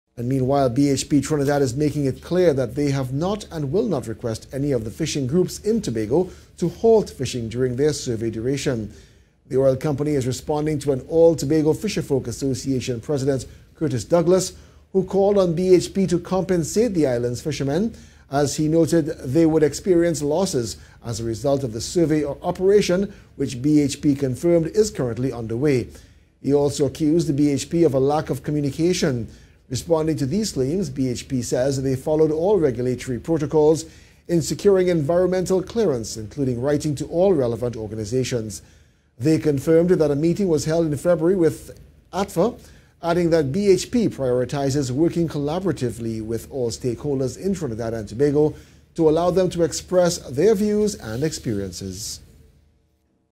BHP Responds to Tobago Fisherfolk Association
BHP Trinidad is making it clear that they have not and will not request any of the fishing groups in Tobago to halt fishing during their survey duration.

The oil company is responding to All Tobago Fisherfolk Association President Curtis Douglas who called on BHP to compensate the island's fishermen, as he noted they would experience losses as a result of the survey operation which BHP confirmed is currently underway.